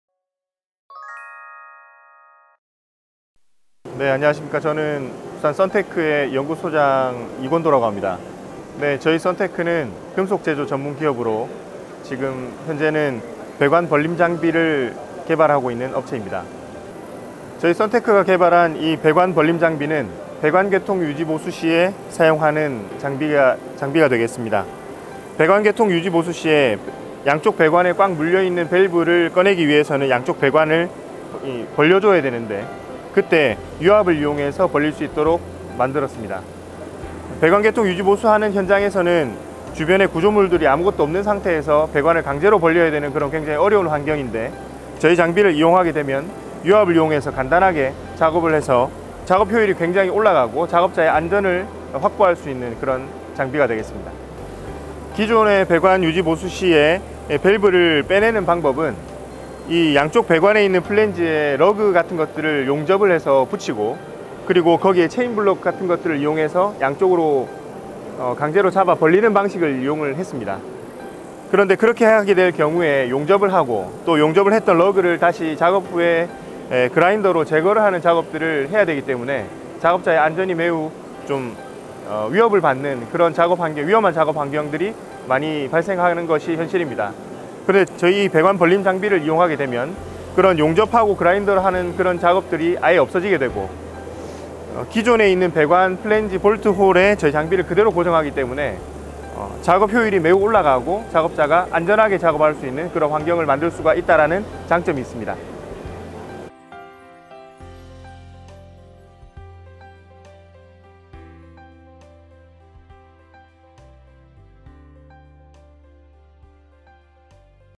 [DAMEX 2023] 썬테크, 배관 개통 유지 보수에 탁월한 '배관 벌림 장비' 출품
썬테크는 레이저 절단, 절곡 등 가공 및 장비 전문 제조 기업으로 배관 벌림 장비를 개발 및 공급하고 있습니다.